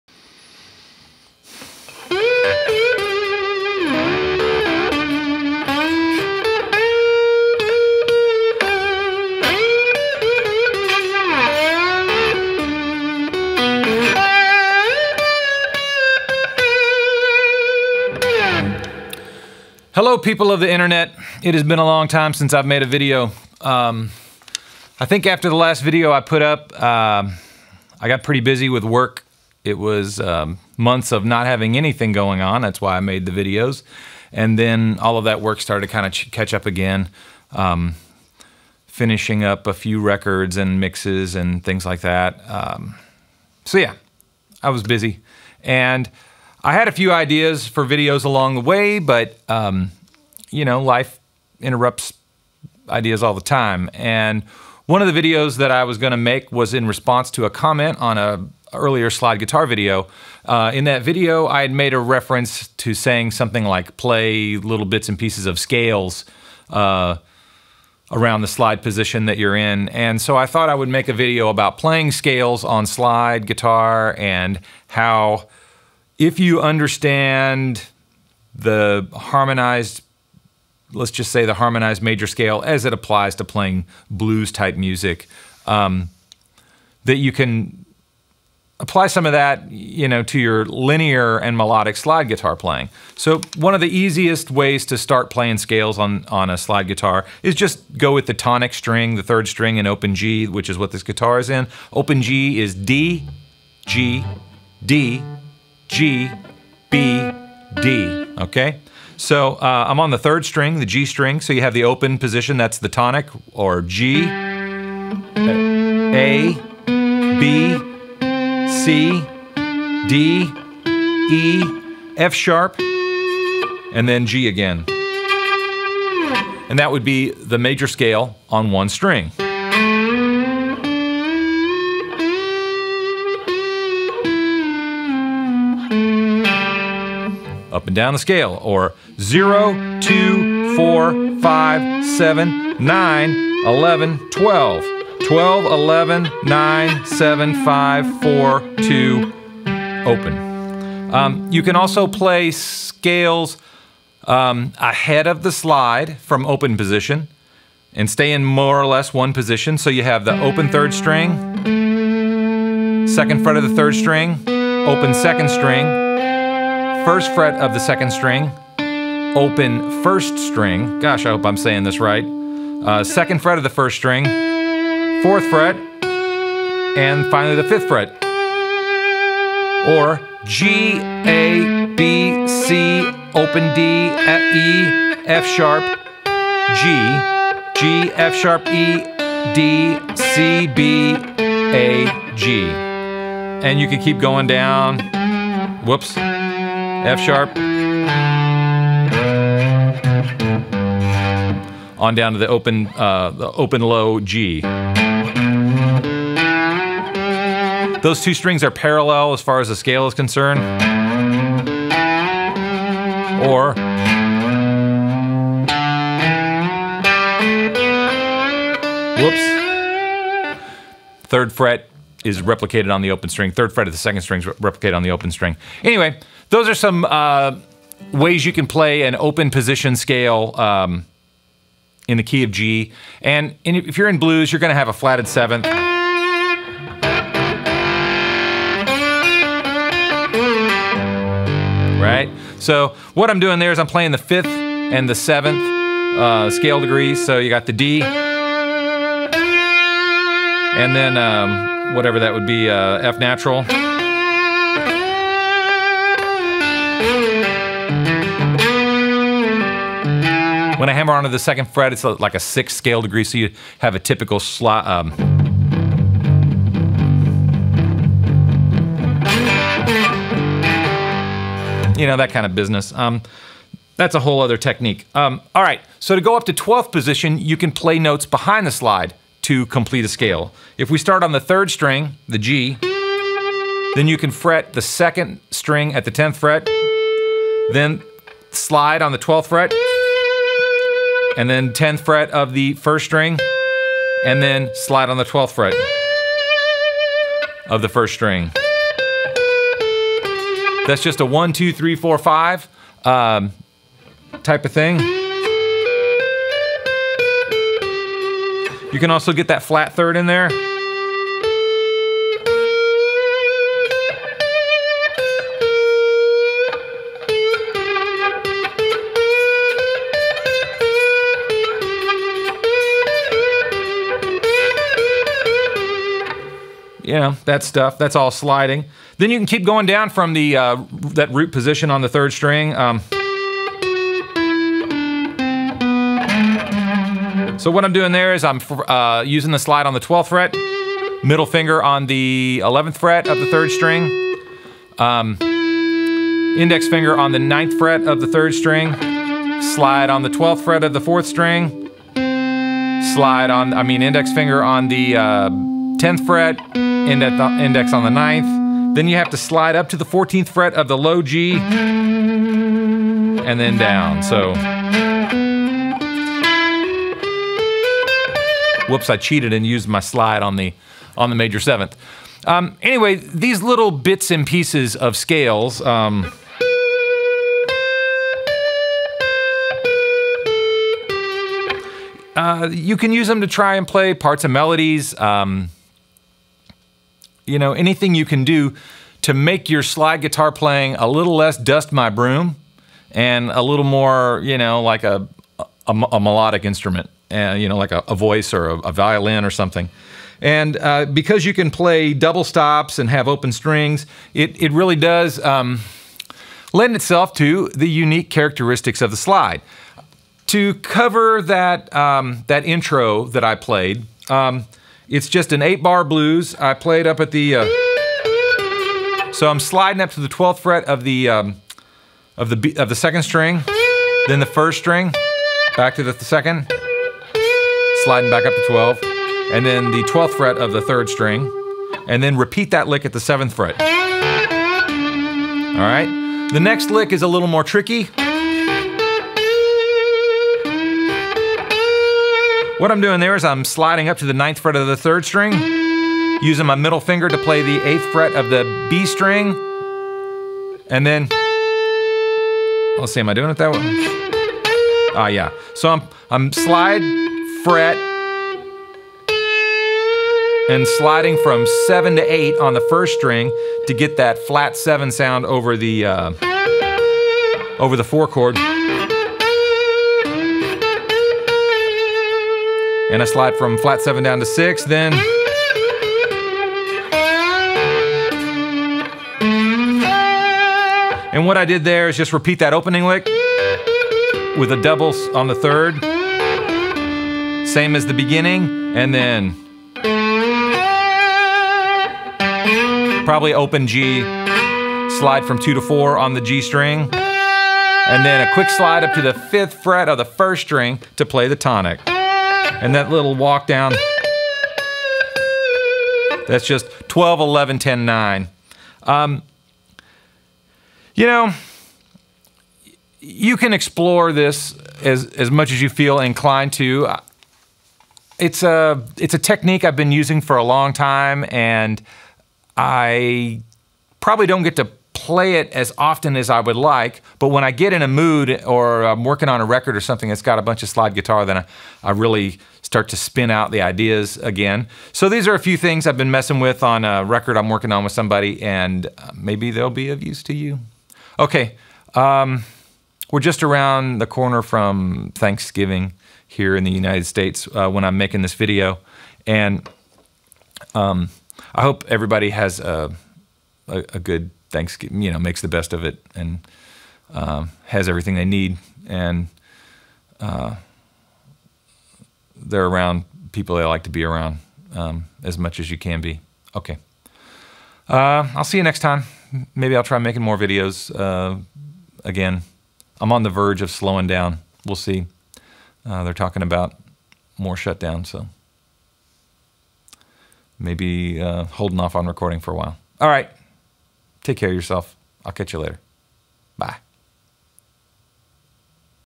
Ep 22 Scales Above and Behind the Slide
In today's video I am demonstrating approaching scaler playing while playing slide guitar. The guitar and amp are both made by Supro in 1959. I'm using a brass slide.